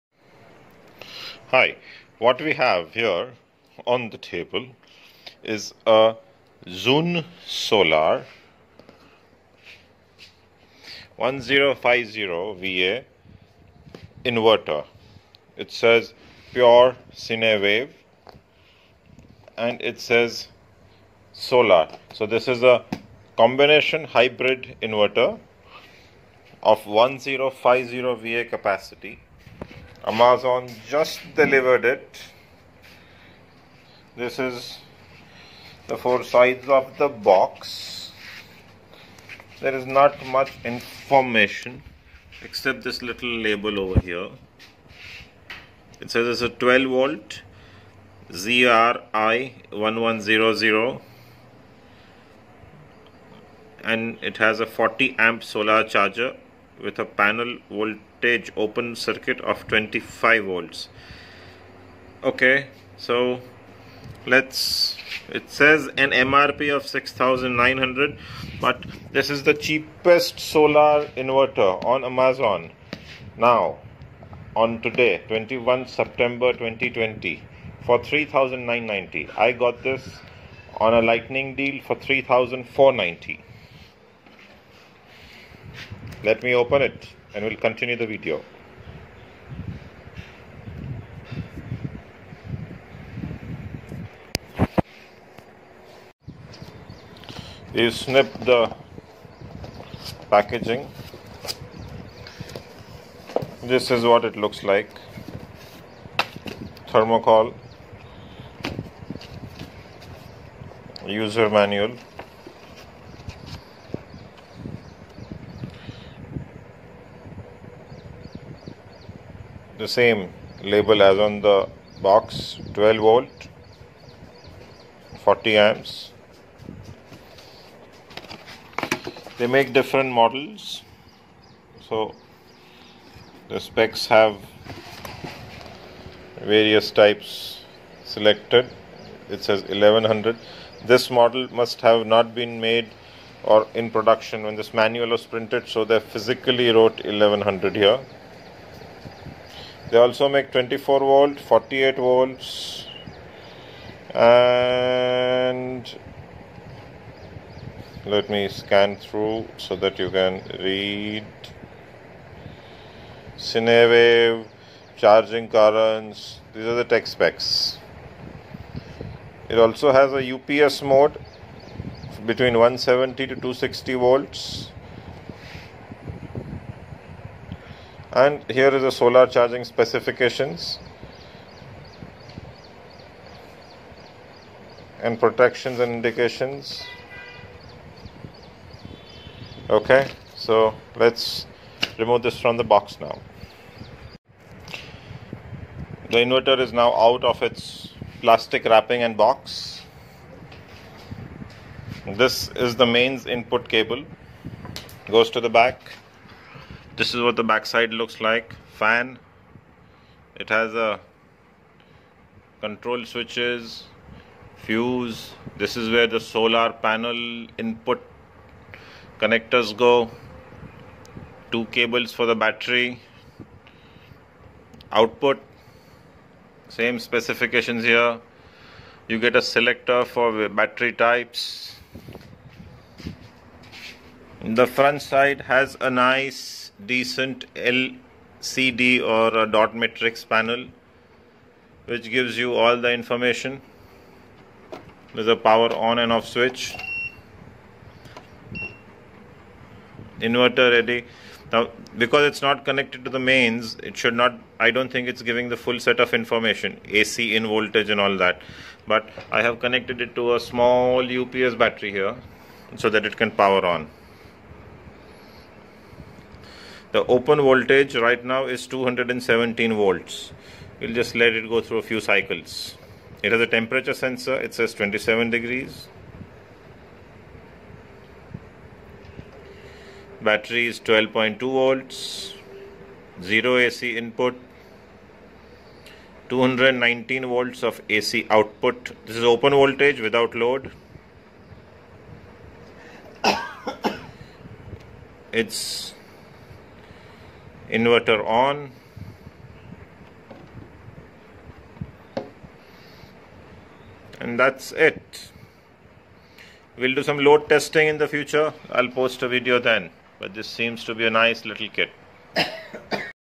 ZunSolar ZRi1100 Solar Hybrid Inverter Unboxing and Startup
Unboxing and Startup of ZunSolar ZRi1100 Pure Sinewave inverter. will post a detailed video after connecting to my solar panels and upload an update to this video.